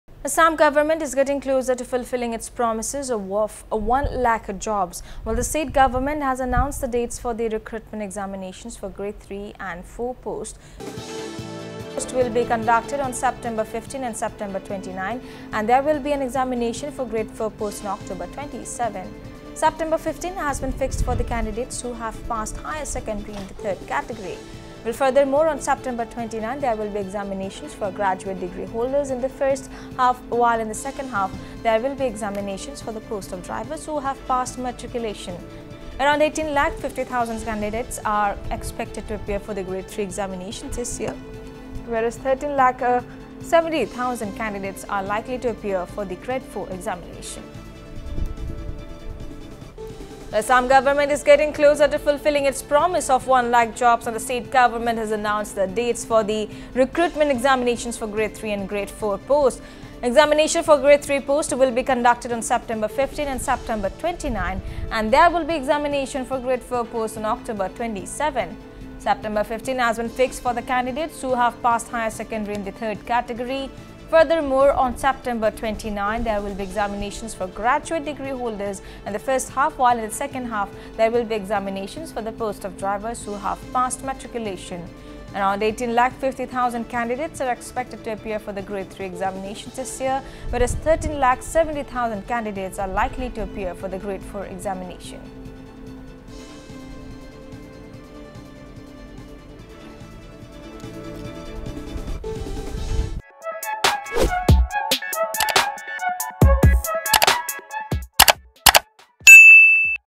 Assam: Government Gets Closer to Fulfilling Promise of 1 lakh jobs
Assam government is getting closer to fulfilling its promise of 1 lakh jobs. The state government has announced the dates for the recruitment examinations for Grade 3 and 4 posts. Examination for Grade 3 posts will be conducted on September 15 and September 29. There will be an examination for Grade 4 posts on October 27. September 15 has been fixed for the candidates who have passed Higher Secondary in the third category. Furthermore, on September 29, there will be examinations for graduate degree holders in the furst half while in the second half, there will be examinations for the posts of drivers who have passed matriculation.